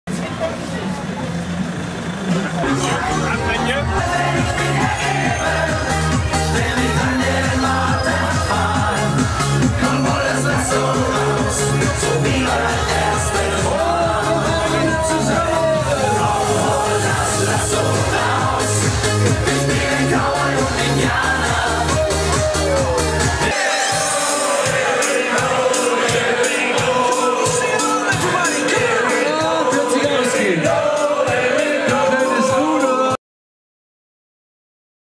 אישגל - סקי דיל - Niki's Apre Ski - SkiDeal
בואו להכיר את חיי הלילה הכי מטורפים בסביבה. באחריות. אישגל, אוסטריה.
למידע נוסף והזמנת חבילות סקי ובטלפון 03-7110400